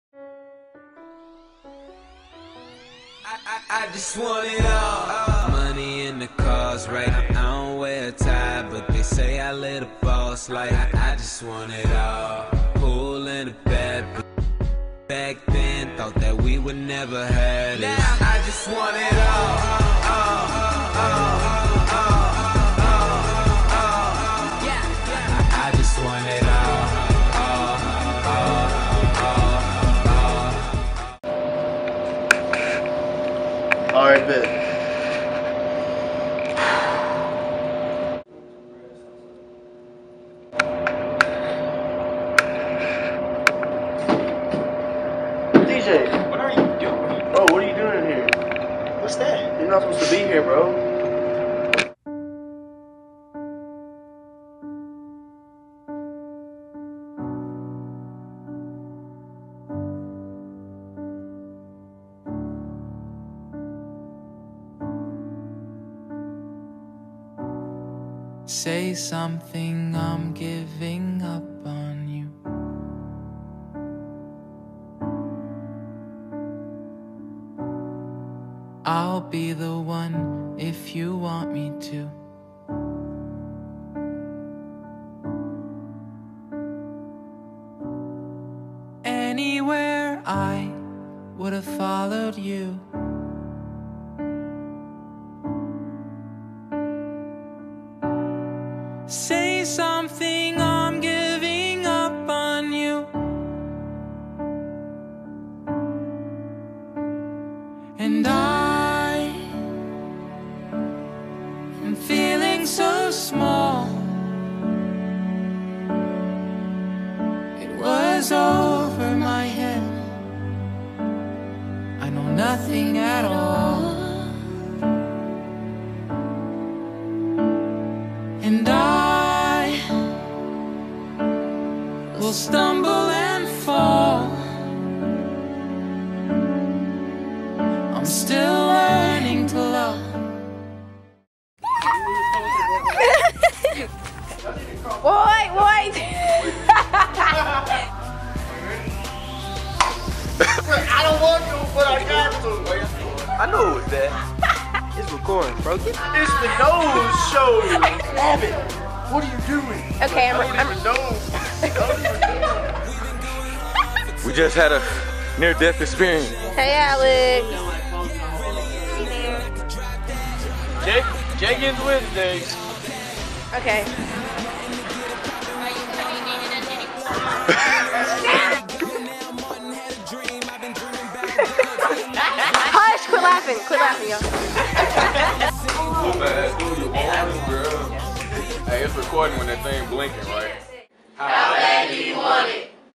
How Bad Do You Want It
A video about students dealing with bullying and drugs. Shot with a Nikon D3100, edited with iMovie.